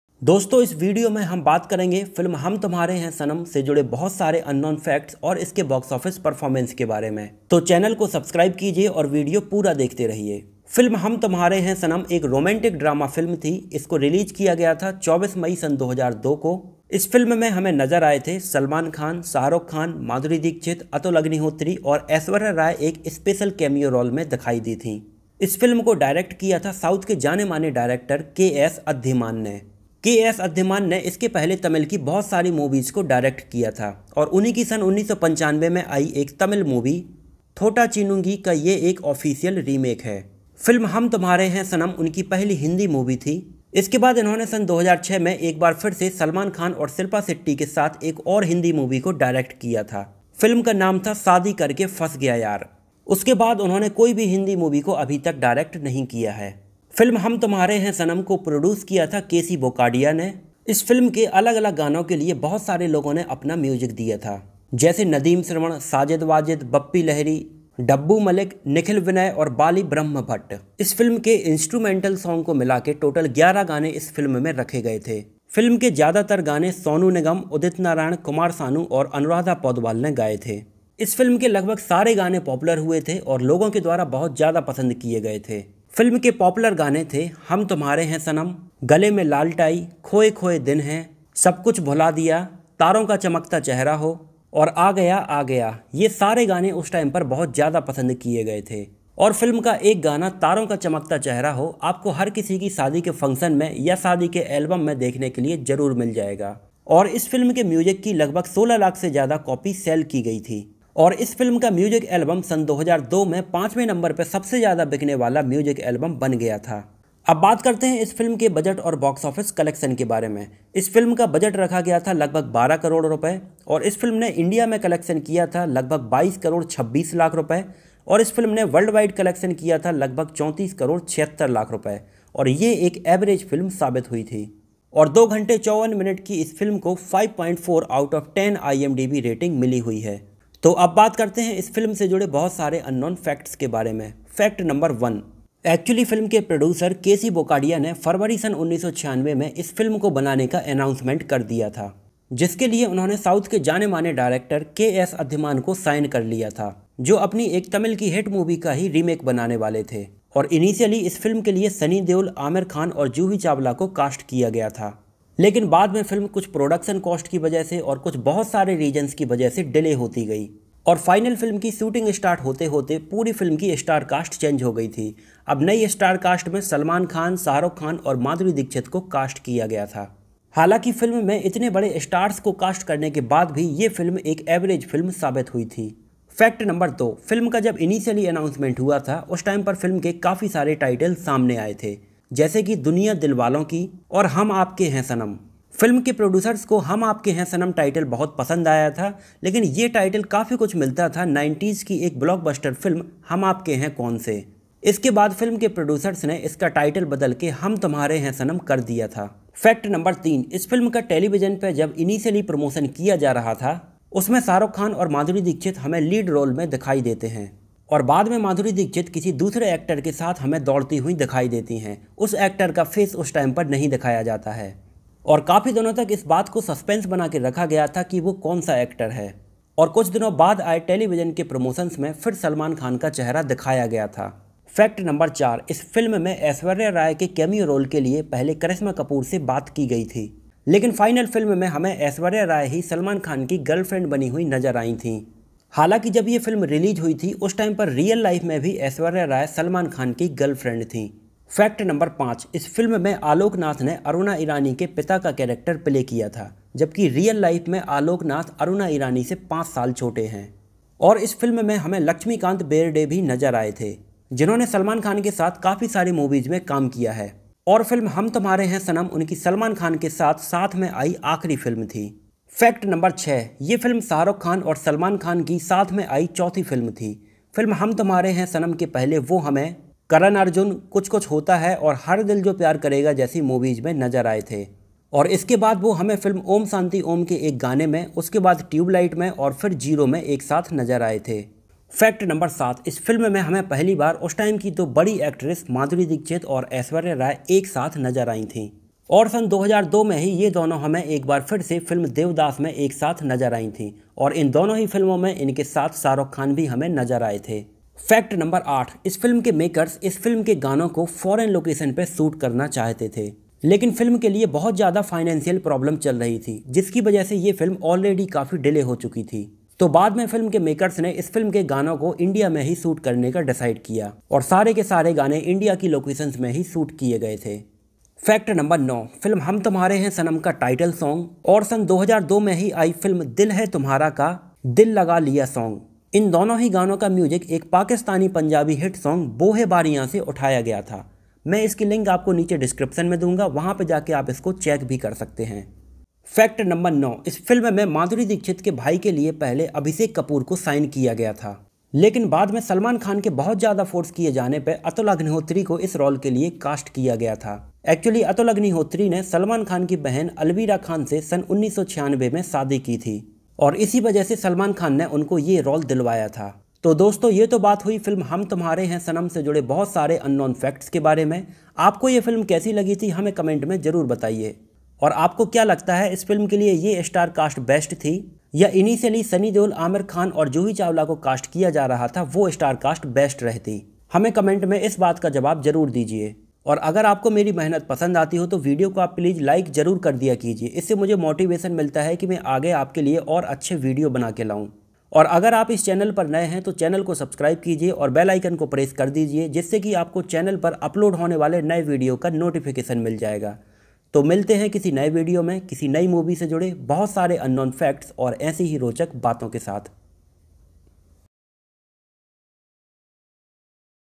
Hum tumhare hain sanam movie unknown facts budget Shahrukh Khan Salman Khan Madhuri dixit movie 2002
Topics covered in this video :
we discuss here 9 unknown facts related to Hum Tumhare hain sanam Bollywood movie 2002

Follw me on Instagram for quick & daily updates
my Instagram Id is svt_14